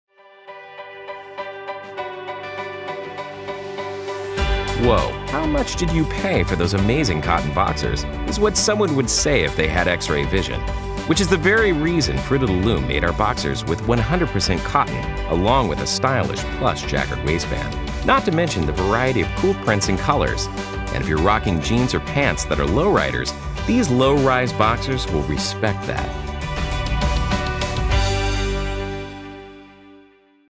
Cotton Men's Boxers by Fruit of the Loom
Made for casual comfort, Fruit of the Loom cotton Men's boxers provide a relaxed fit. A variety of prints ensure you will find a design that fits your style.